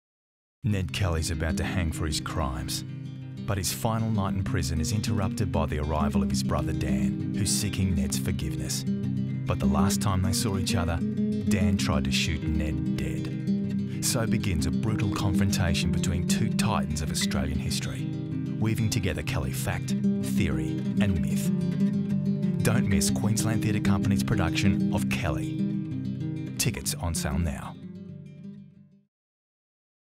KELLY - Touring Video
Ned Kelly sits in his prison cell. Tomorrow he is going to hang. A wounded beast in a cage, defiant till the end. A priest enters to give him his last rites and it is revealed to be Dan Kelly - Ned's younger brother believed burnt to death at Glenrowan. Dan is desperate and needs one last thing from his older brother and Ned stands firmly against him.